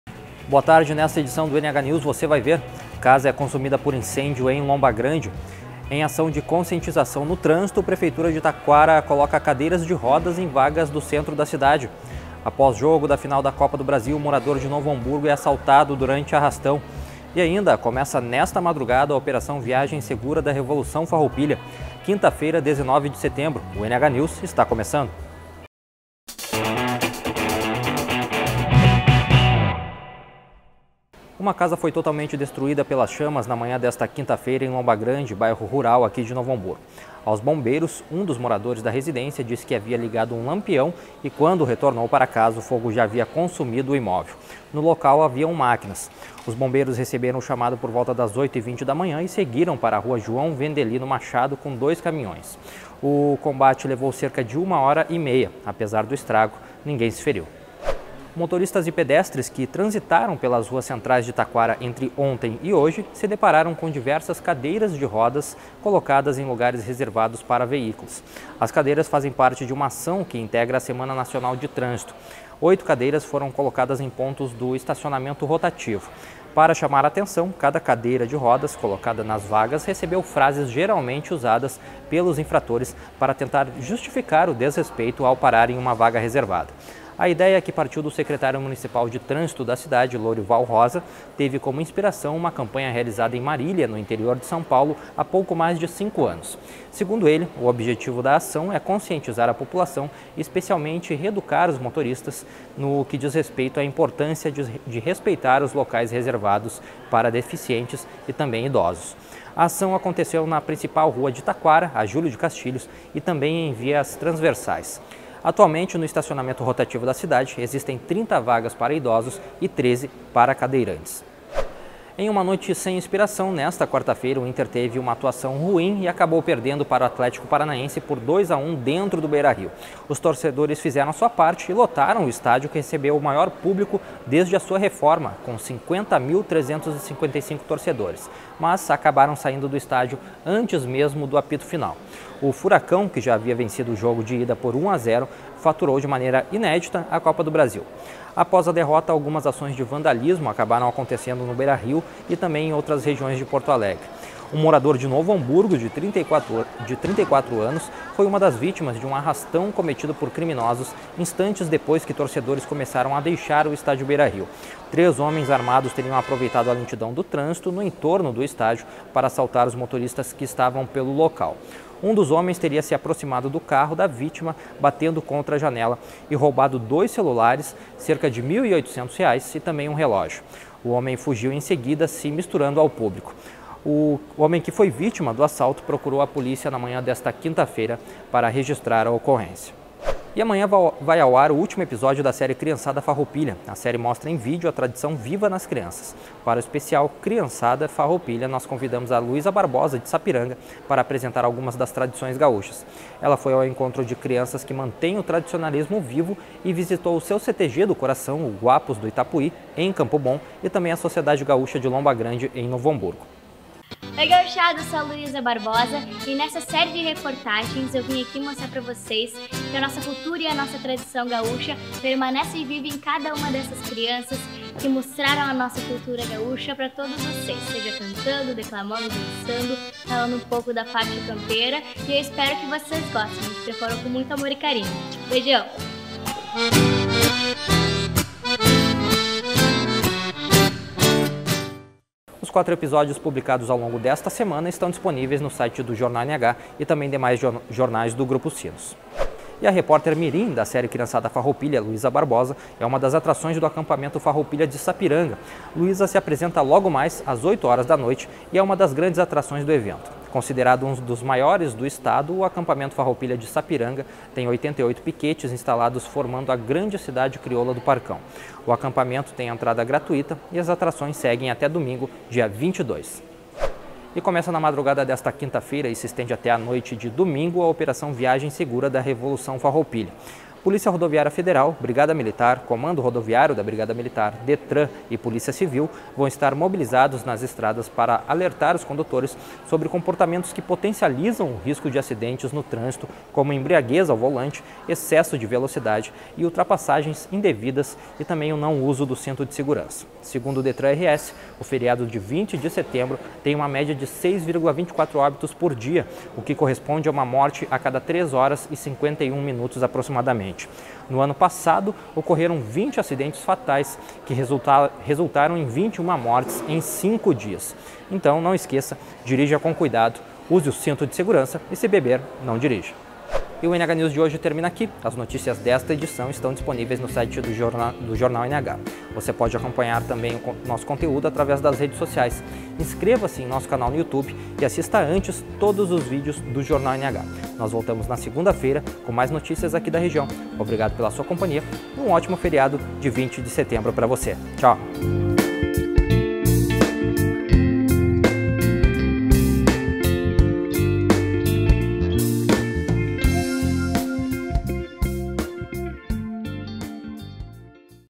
📺 NH News: prefeitura de Taquara coloca cadeiras de rodas em vagas de carros
Veja também: Casa é consumida por incêndio em Lomba Grande.